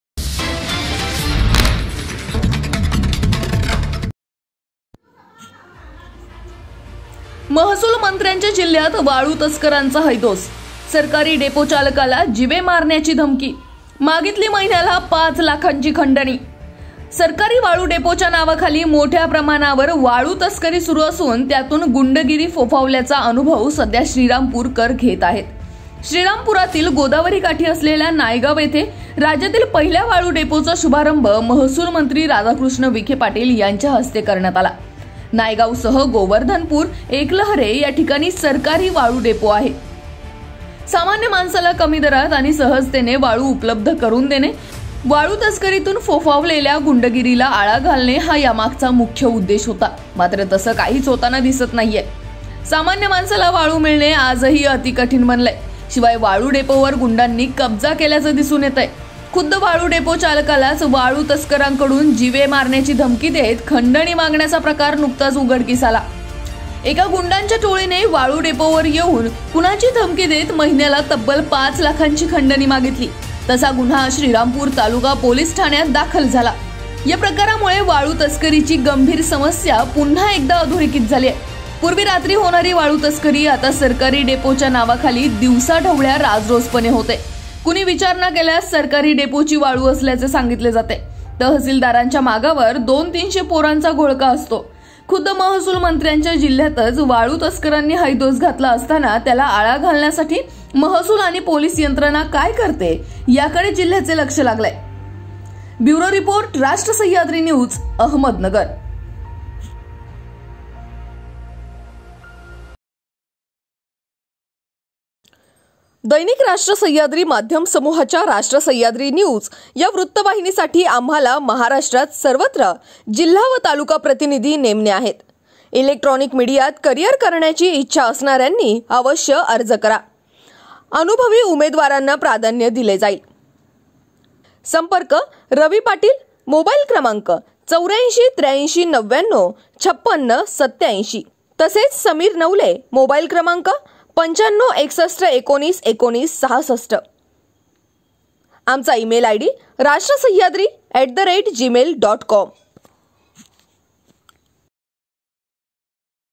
महसूल मंत्र्यांच्या जिल्ह्यात वाळू तस्करांचा हैदोस..
सरकारी वाळू डेपोच्या नावाखाली मोठ्या प्रमाणावर वाळू तस्करी सुरू असून त्यातून गुंडगिरी फोफावल्याचा अनुभव सध्या श्रीरामपूरकर घेत आहेत.

श्रीरामपुरातील गोदावरी काठी असलेल्या नायगाव येथे राज्यातील पहिल्या वाळू डेपो चा शुभारंभ महसूल मंत्री राधाकृष्ण विखे पाटील यांच्या हस्ते करण्यात आला. नायगावसह गोवर्धनपुर, एकलहरे या ठिकाणी सरकारी वाळू डेपो आहेत.
सामान्य माणसाला कमी दरात आणि सहजतेने वाळू उपलब्ध करून देणे, वाळू तस्करीतून फोफावलेल्या गुंडगिरीला आळा घालणं हा यामागचा मुख्य उद्देश होता.
मात्र तसं काहीच होताना दिसत नाही. सामान्य माणसाला वाळू मिळणे आजही अति कठीण बनले आहे. शिवाय वाळू डेपोवर गुंडांनी कब्जा केल्याचे दिसून येतंय. खुद्द वाळू डेपो चालकालाच वाळू तस्करांकडून जीवे मारण्याची धमकी देत खंडणी मागण्याचा प्रकार नुकताच उघडकीस आला.
एका गुंडाच्या टोळीने वाळू डेपो चालत आला कुणाची धमकी देत महिन्याला तब्बल पाच लाखाची खंडणी मागितली. तसा गुन्हा श्रीरामपूर तालुका पोलीस ठाण्यात दाखल झाला.